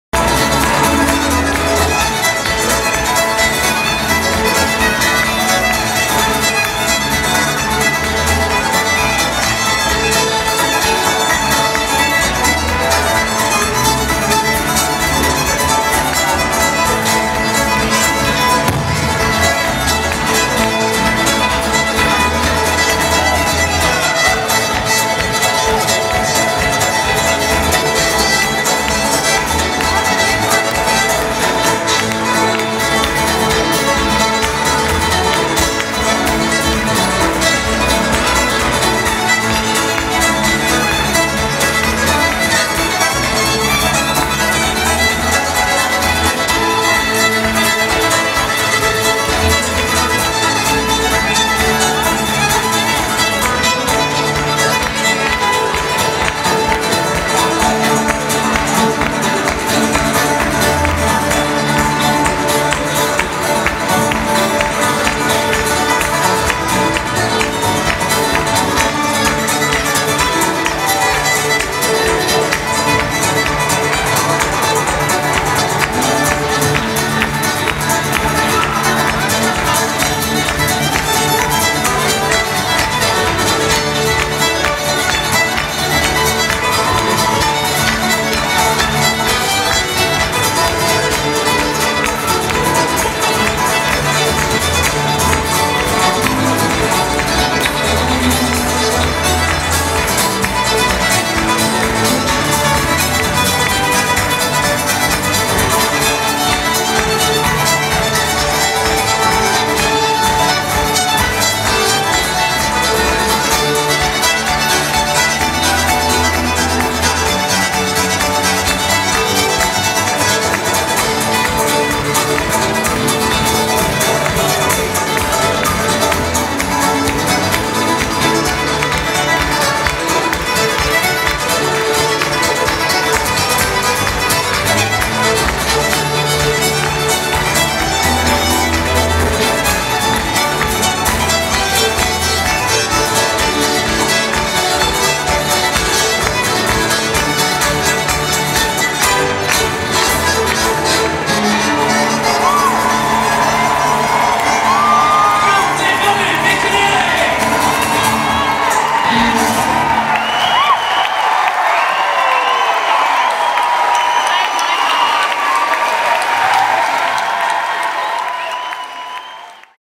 We Banjo 3 with David Munnelly & Mick Conneely @ KC Irish Fest 2013
We Banjo 3 invited David Munnelly and Mick Conneely onto the stage during their final set of Kansas City Irish Fest 2013. The group and their guests had much of the crowd up and dancing.